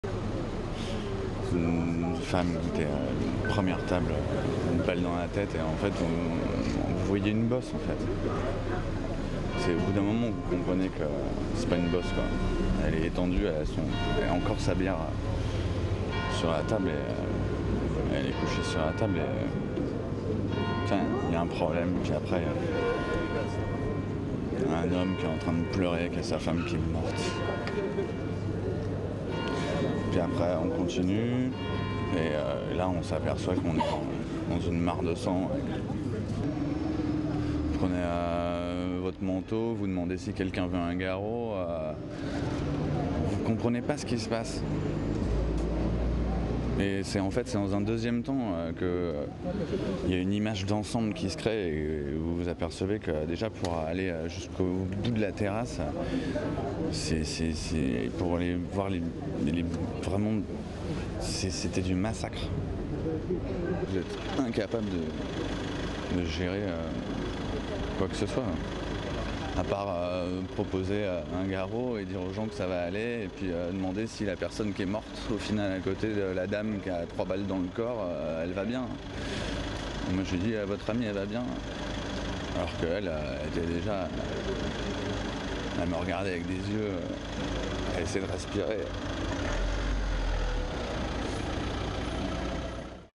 Attentats de Paris : un témoin de la rue de Charonne raconte
PARIS - 14 nov 2015 - Plus de 18 personnes sont décédées rue de Charonne dans l'attaque de plusieurs terrasses vendredi soir. Sébastien Jagreau présent au moment de la fusillade témoigne.
Les attentats perpétrés dans six lieux différents à Paris et Saint-Denis ont causé la mort de plus de 120 personnes.